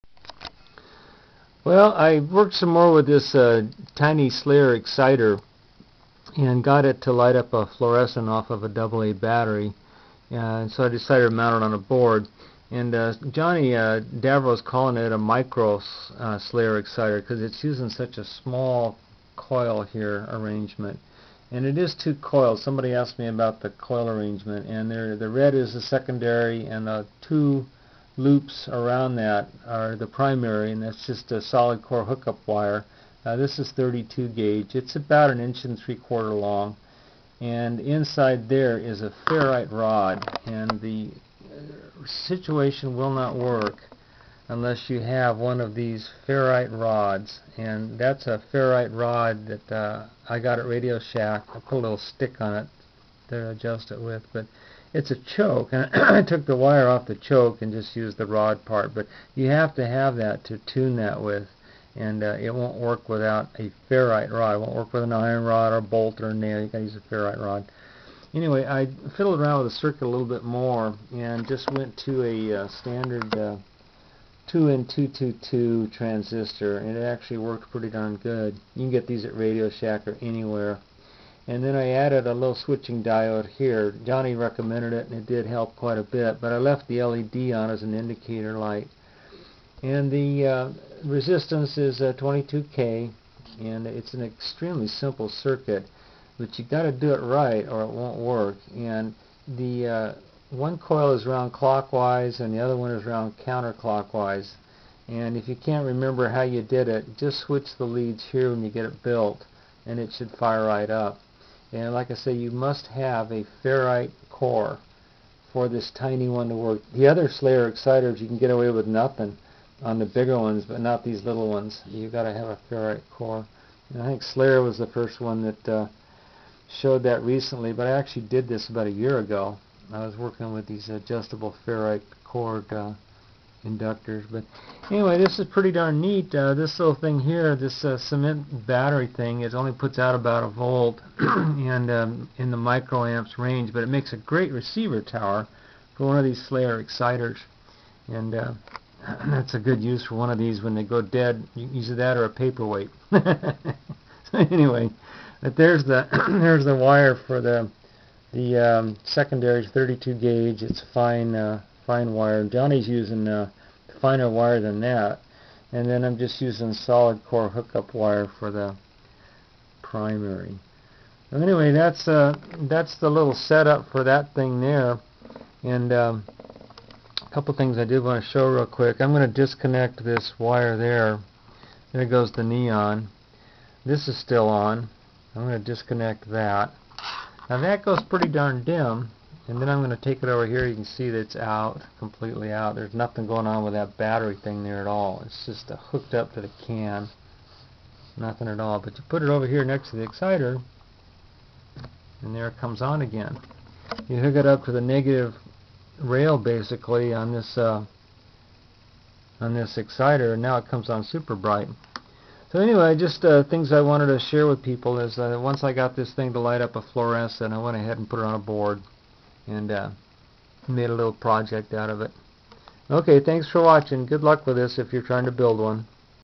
Tiny Slayer Exciter mounted on a circuit board.ASF
This is a  tiny Slayer Exciter mounted on a circuit board.  I show it here lighting up a 4 watt fluorescent tube as well as a neon bulb and three LEDs.  The circuit is being run on one AA battery.  This basic Slayer Exciter circuit diagram is shown.  More information on the miniaturization of this circuit  can found at Jonnydavro's channel.  GBluer (Slayer 007) here at Youtube is the original inventor of the circuit and many videos showing many variations are available at his channel.